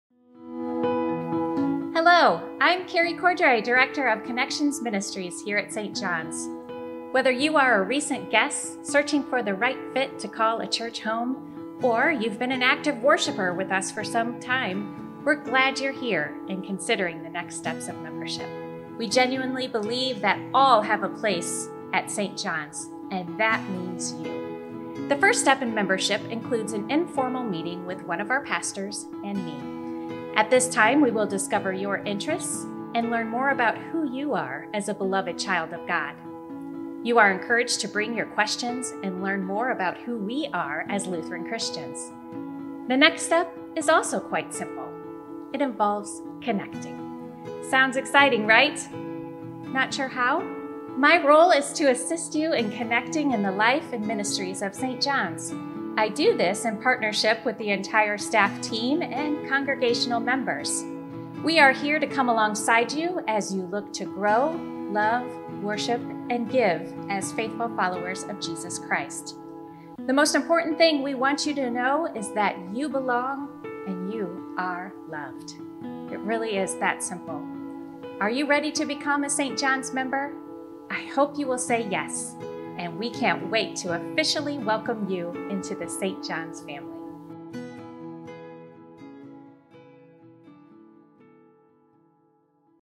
Member Connections
Learn about Member Connections from Kerri Cordray, Director of Connections.